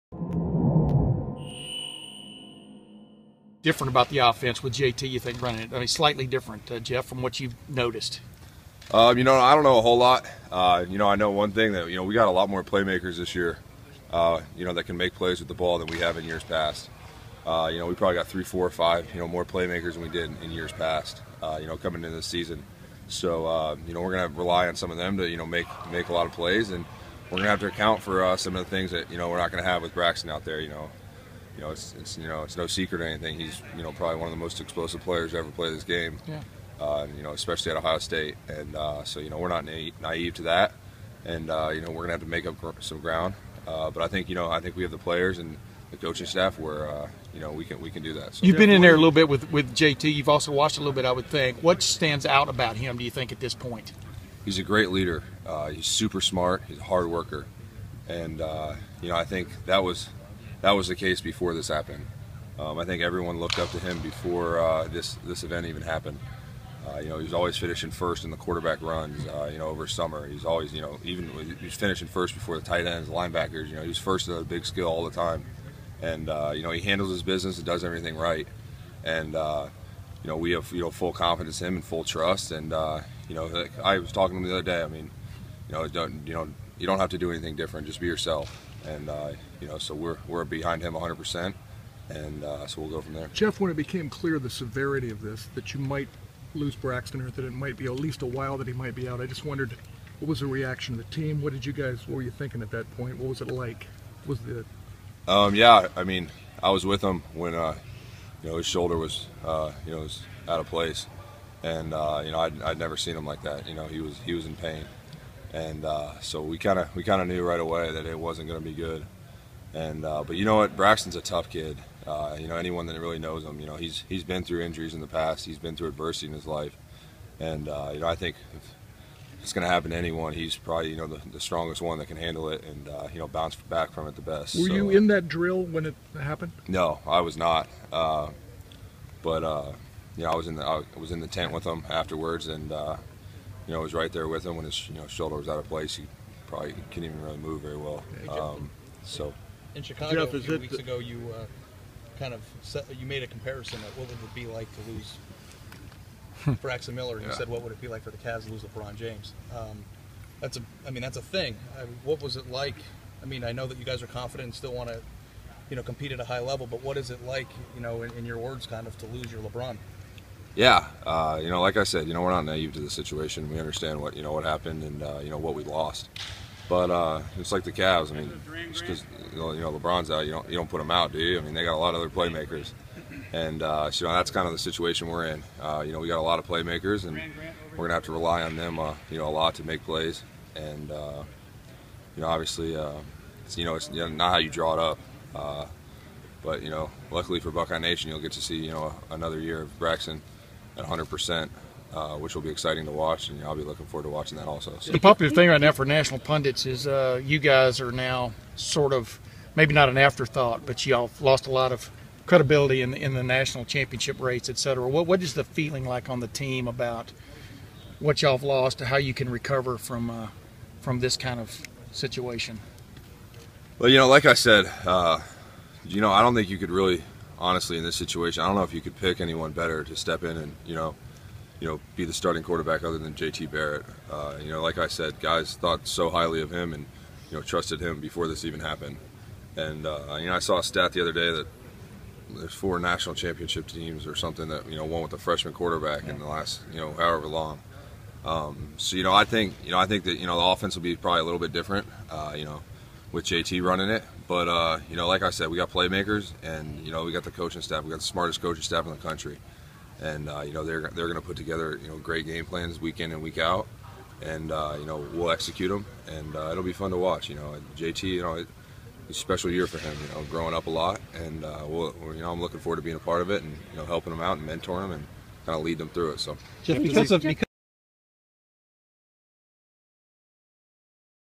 Jeff Heuerman addresses Braxton Miller's injury
Ohio State senior tight end Jeff Heuerman said in late July that losing Braxton Miller would be the equivalent of the Cleveland Cavaliers losing LeBron James. Now that Miller is out for the year, Heuerman reacts to that comment.